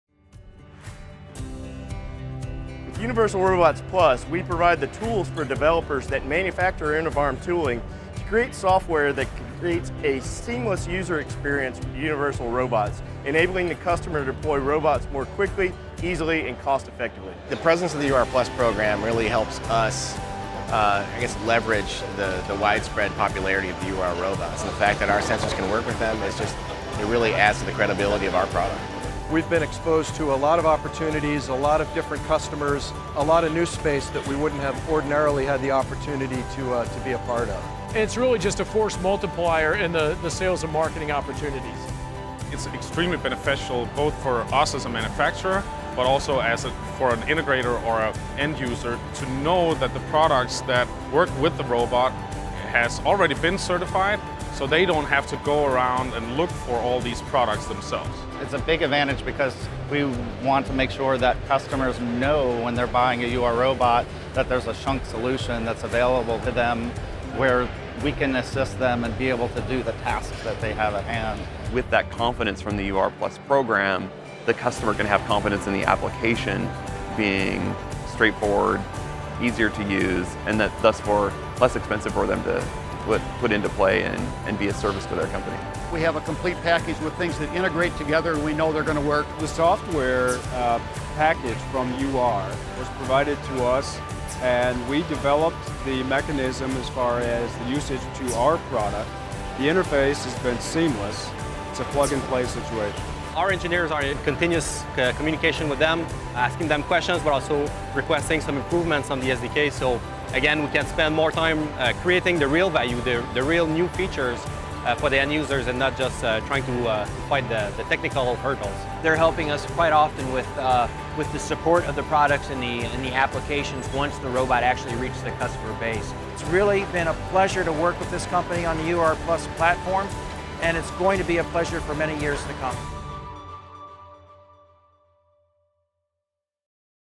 Save time and money with cutting-edge products for your cobot
UR+ is the premier product platform to help you automate your applications more easily than ever before. Hundreds of industry-leading products are tested and approved to work with cobots from Universal Robots to provide you with fast deployment, simple programming and reliable operation. From grippers and accessories to vision systems and software – UR+ has a solution for you.